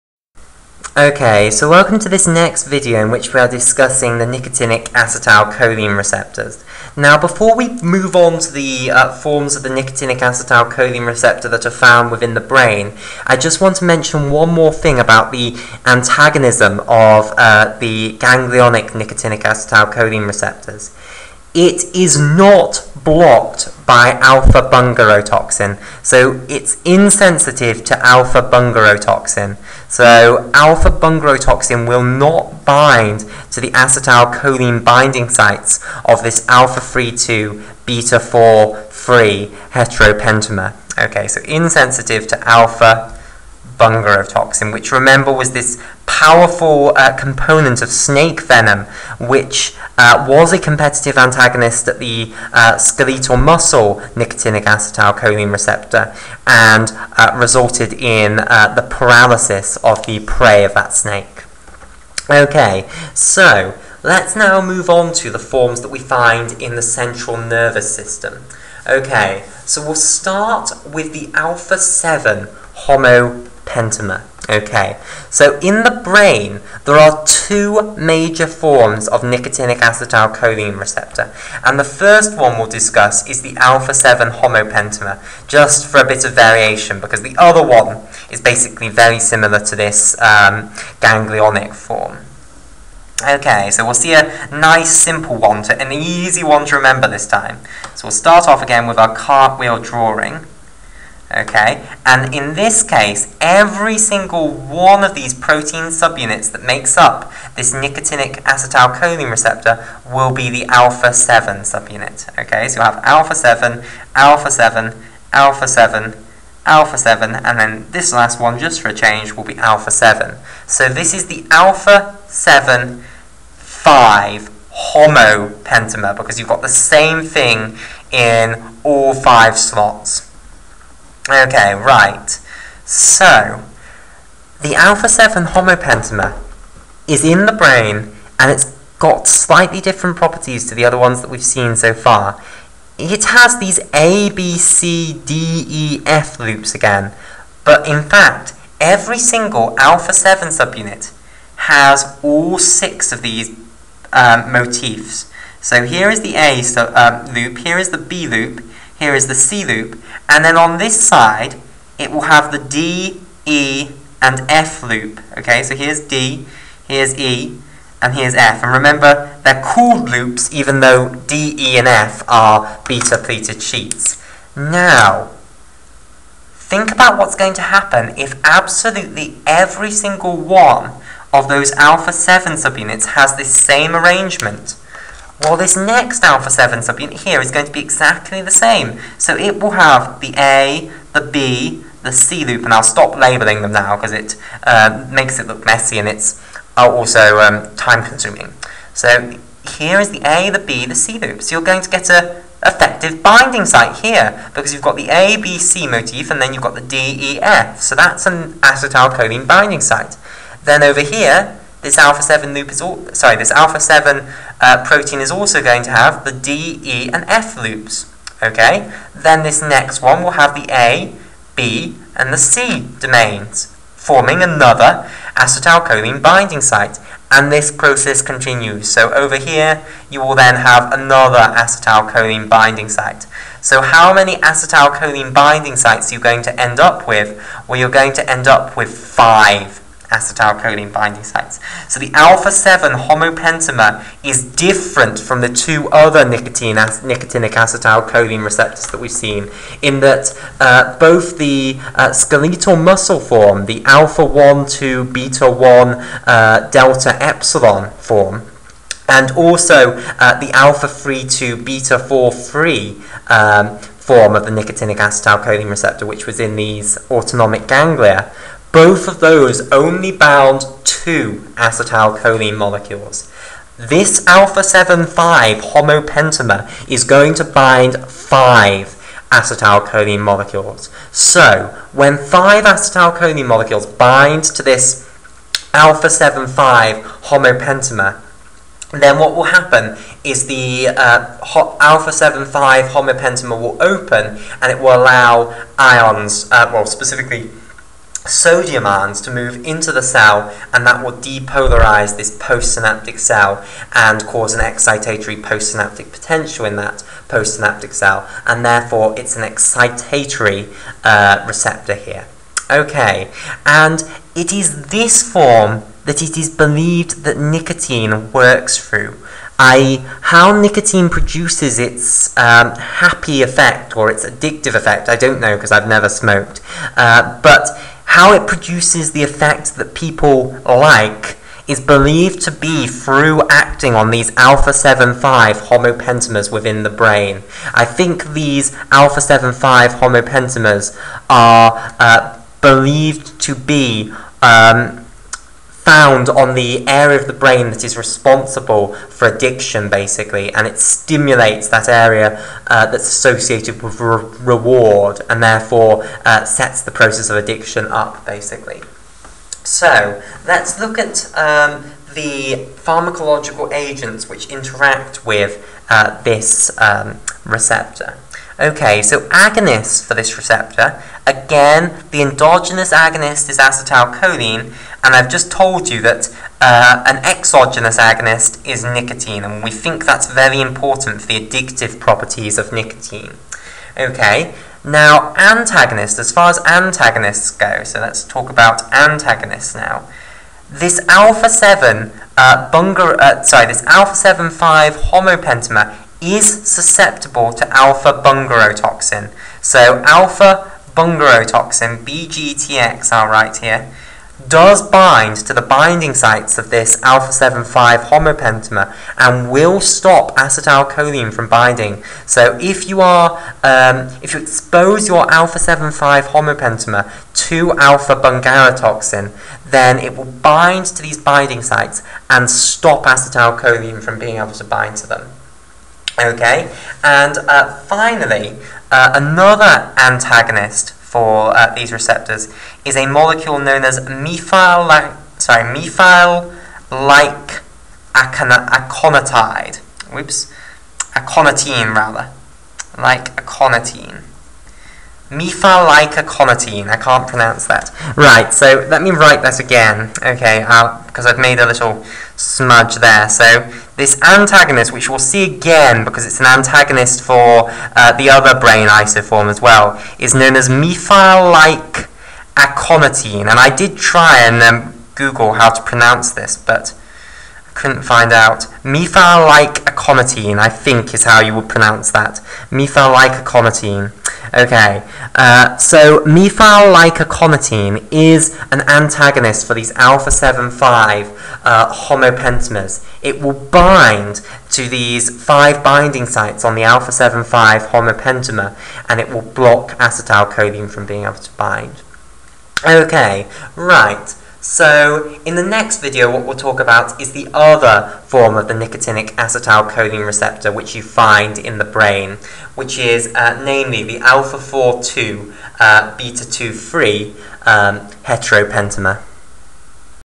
Introduction to Nicotinic Acetylcholine Receptors Part 4
In this video we discuss the subunits of the nicotinic acetylcholine receptors and look at the subunit composition of the major types of nicotinic acetylcholine receptor.